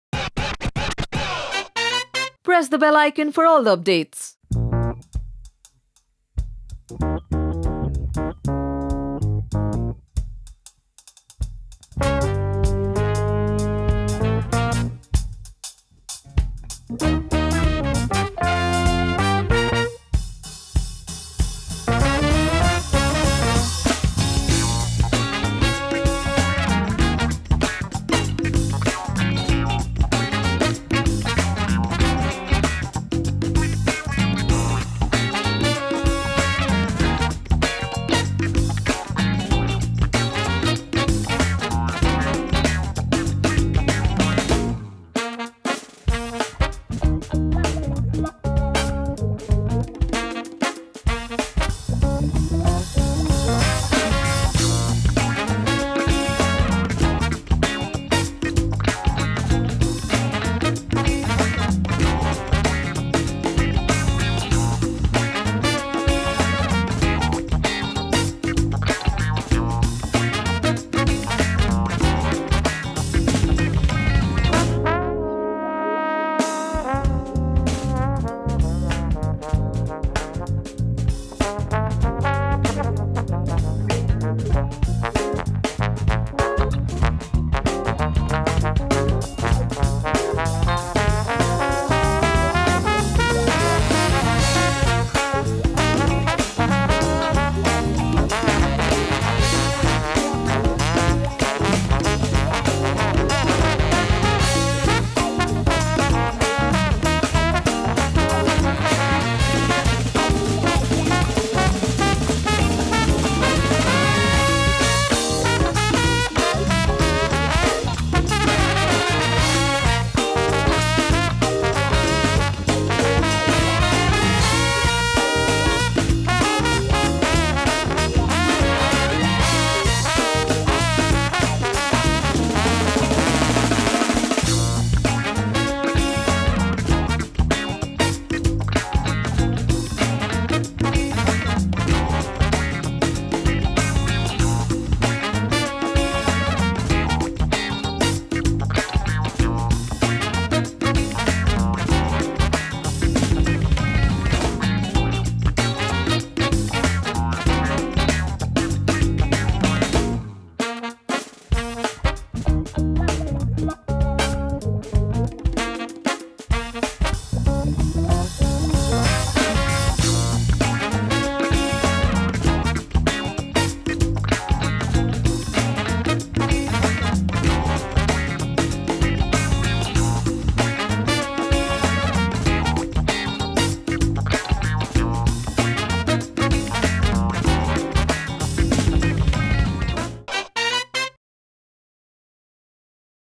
Hiba Nawab And Nikhil Khurana Pool Masti | Jijaji Chhat Par Hai
Watch the video for Hiba Nawab and Nikhil Khurana pool masti as they shoot the flood sequence for the upcoming episodes of Jijaji Chhat Par Hai.

Reporter : Neha Antani
Cameraman : Saagar Choudhary
Editor : Amit Gupta.